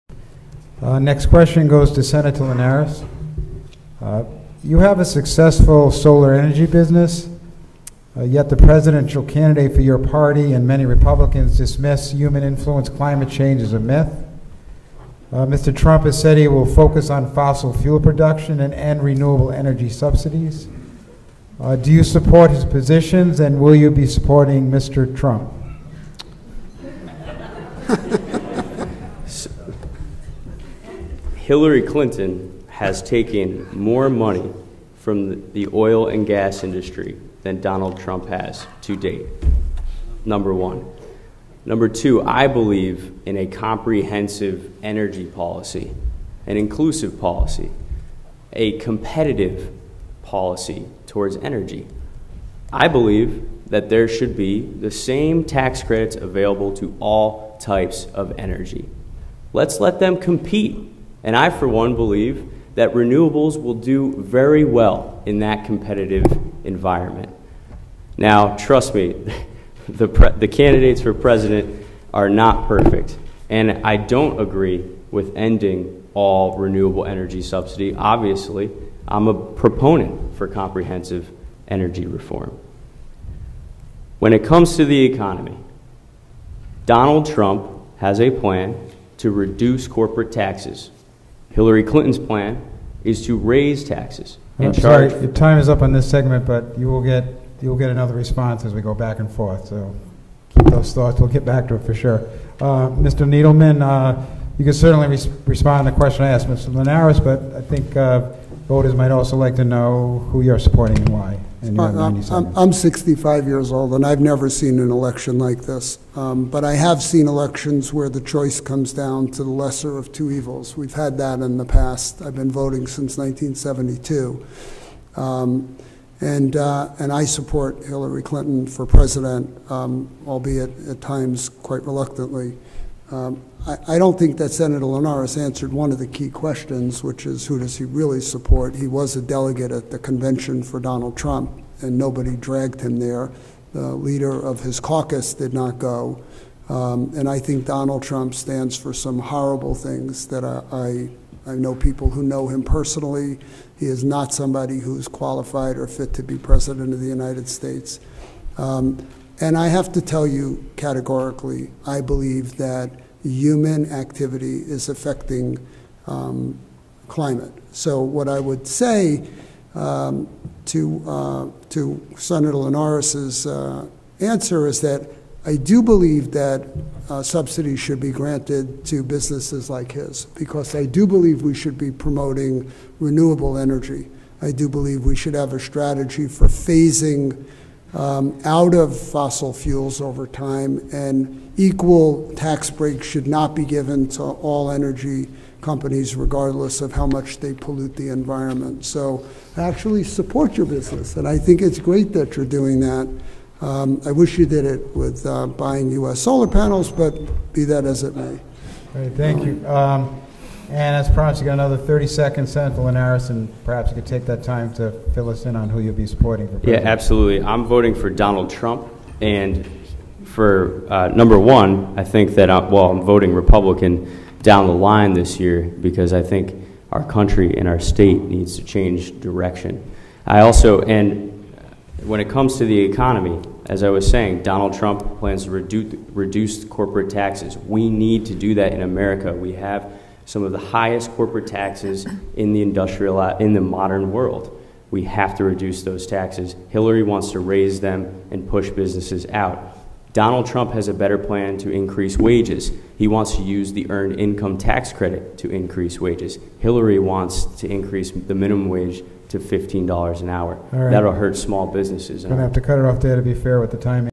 8. Do you support Trump’s position on renewable energy and his candidacy?
Incumbent Republican Sen. Art Linares and Democrat Norm Needleman are running for the state Senate seat representing Connecticut's 33rd District.